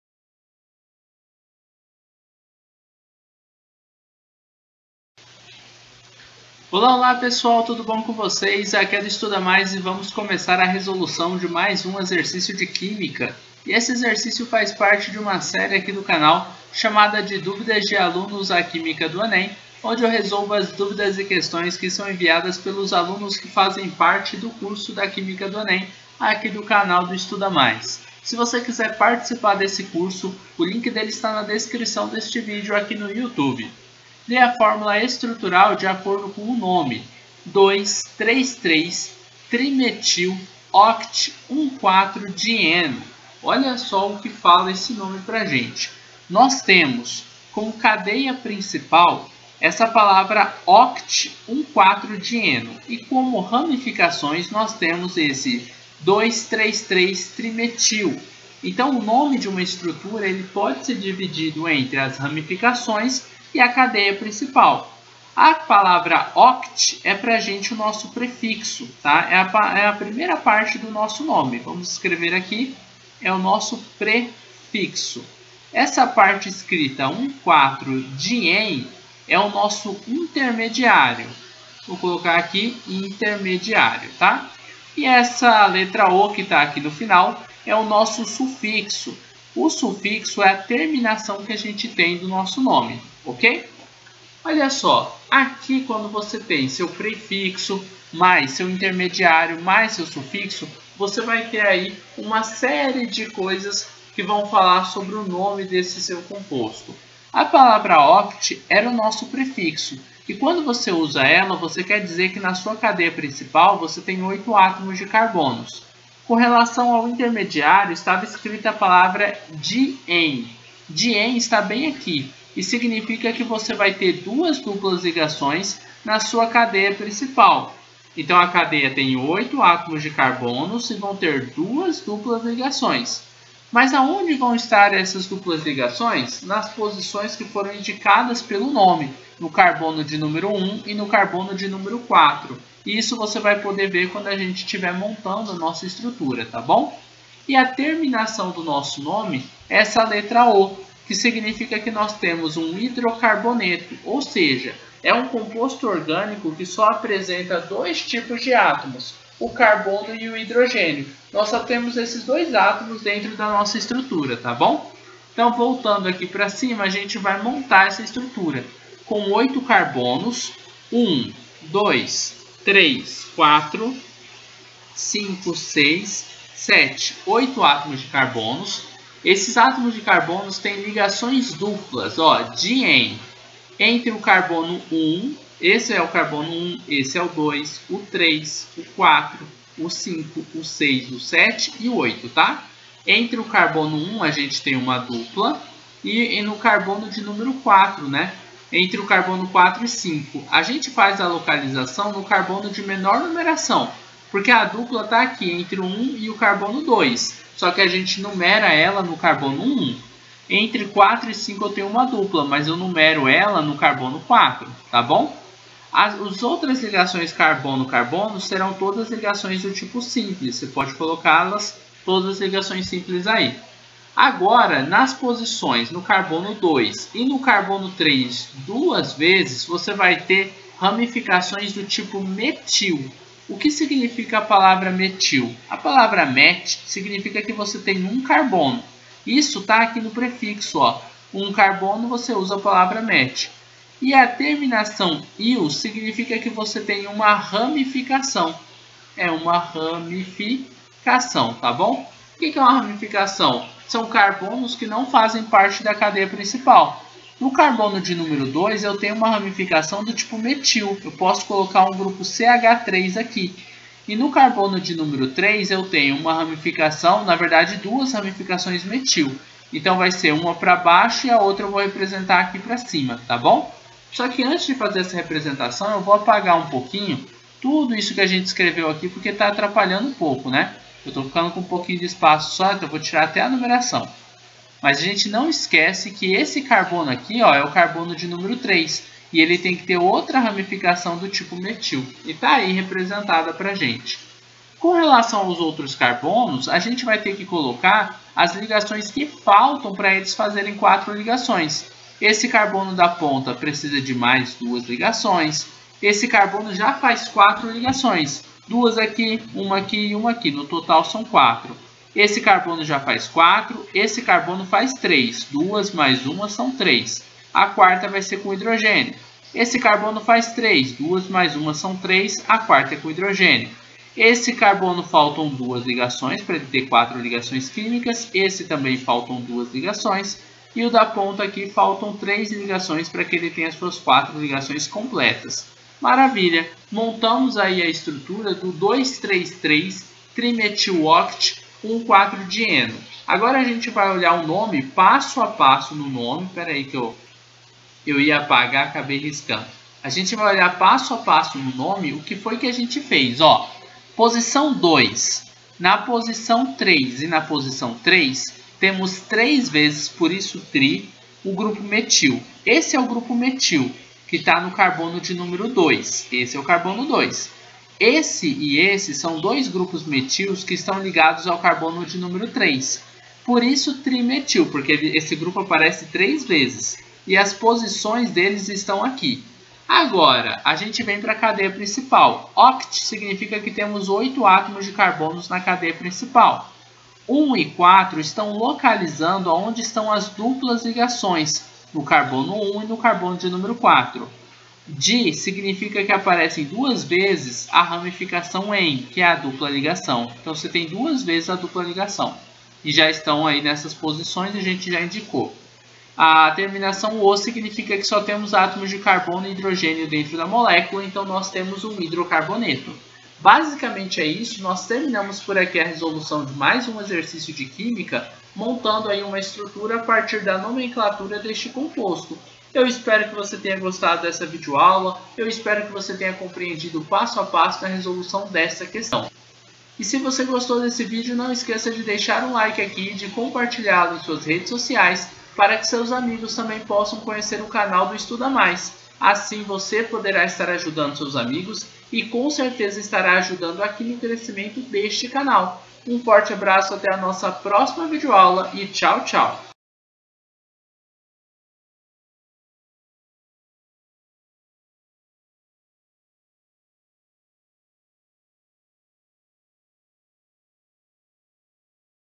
Dê a fórmula estrutural de acordo com o nome: 2,3,3-trimetiloct-1,4-dieno
Dê a fórmula estrutural de acordo com o nome:
2,3,3-trimetiloct-1,4-dieno